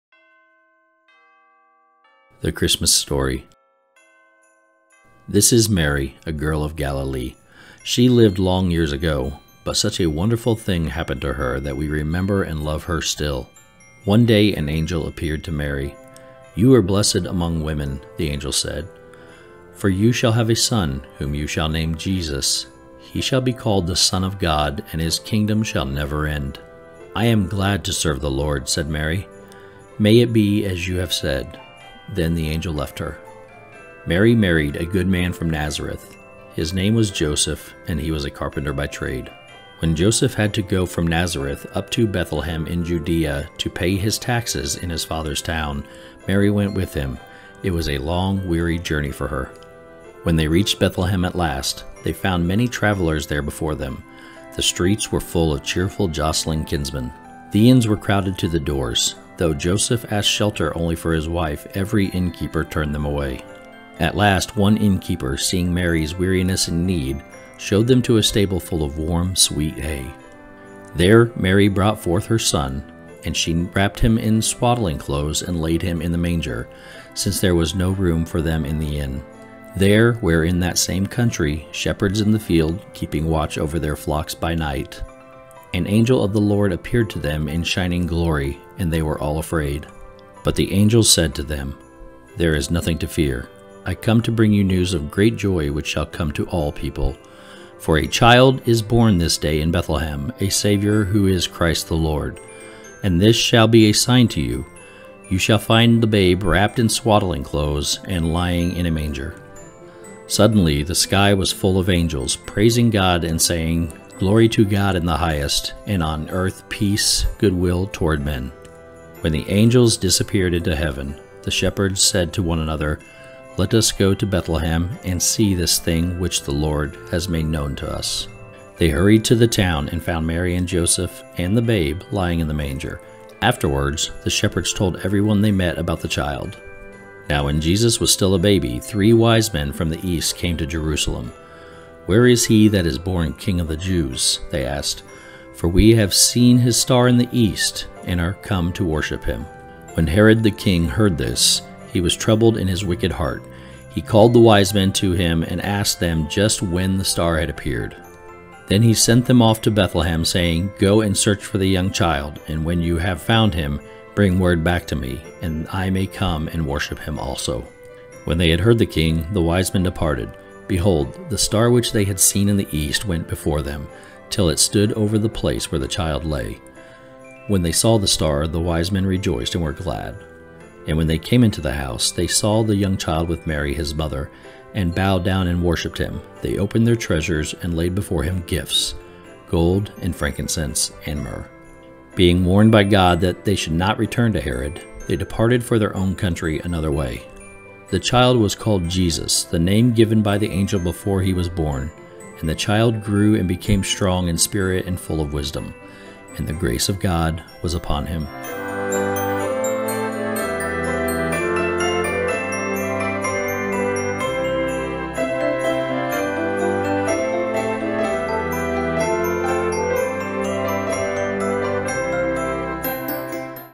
Vlogmas Day 25 | The Christmas Story
This is a difficult task and I encourage you to check out other creators for the work they have put in. This is my third Vlogmas since I joined YouTube and I have themed the previous 2. This year my intentions are to read Christmas stories with illustrations. Today's Vlogmas reading is The Christmas Story!